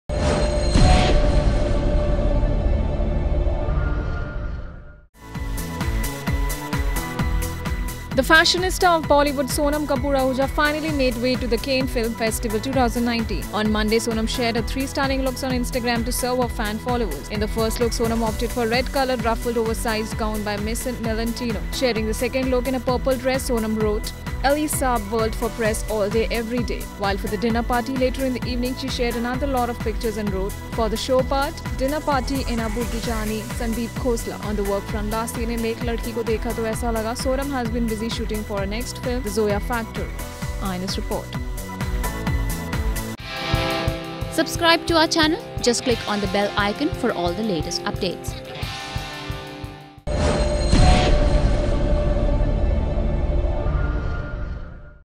Sonam K Ahuja poses for three stunning looks at Cannes 2019
The fashionista of Bollywood Sonam Kapoor Ahuja finally made way to the Cannes Film Festival 2019. On Monday, Sonam shared her three stunning looks on Instagram to serve her fan followers.